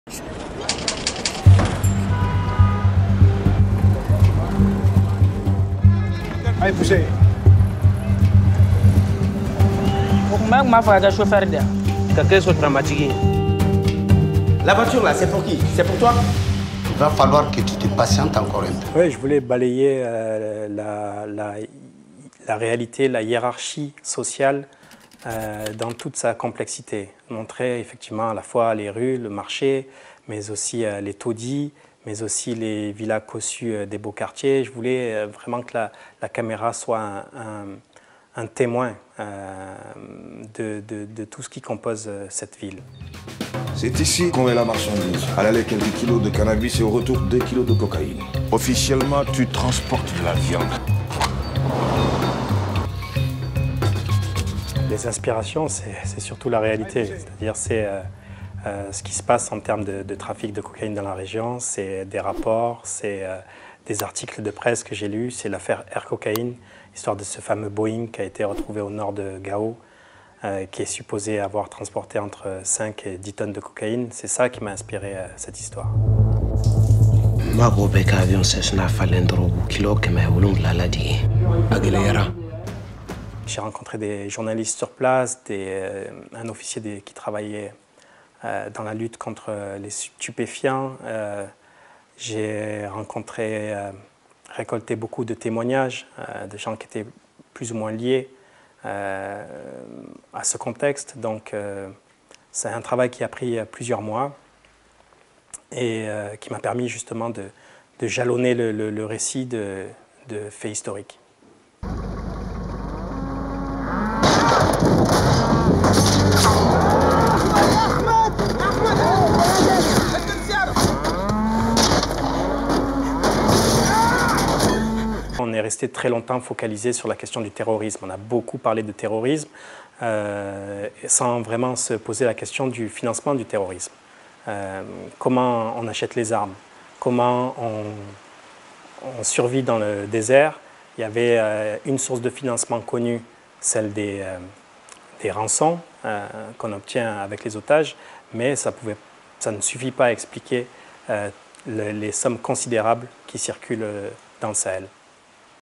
[Archive] Rencontre avec le réalisateur franco-malien du film Wùlu
Wùlu est le premier long-métrage du réalisateur franco-malien Daouda Coulibaly. Ce thriller met en lumière les trafics de cocaïne qui rongent le Mali et contribuent au financement du terrorisme. À travers son film, le cinéaste voulait "balayer la réalité, la hiérarchie sociale dans toute sa complexité, que la caméra soit un témoin". Pour la préparation de Wùlu, Daouda Coulibaly s'est inspiré du trafic dans la région mais aussi de l'affaire Air Cocaïne, le fameux Boeing retrouvé au nord du pays en 2009 et qui aurait servi à transporter entre 5 et 10 tonnes de cocaïne.